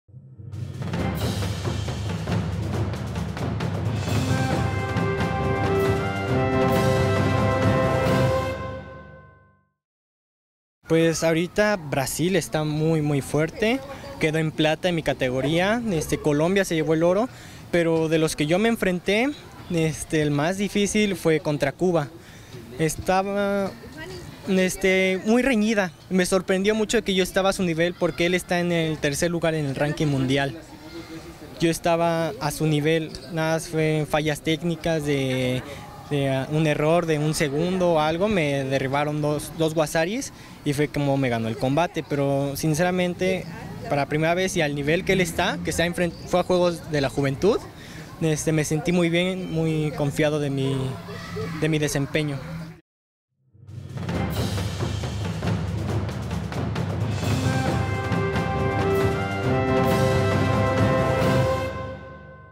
Brandon González participará en el mundial de judo sub 18 en Kazajistán
El judoca veracruzano Brandon González Rojas esta por participar en el mundial de judo para cadetes sub 18 en Kazajistán representando a México dentro de la división de hasta 65 kilogramos. Brandon tras participar en el campeonato panamericano de la especialidad logró conseguir su pase en la posición número 7 de 8 participantes dandole batalla a los grande exponente de está categoría a nivel continental. En la charla con los micrófonos de Más Deportes nos comparte los rivales que establecieron condiciones dentro del panamericano.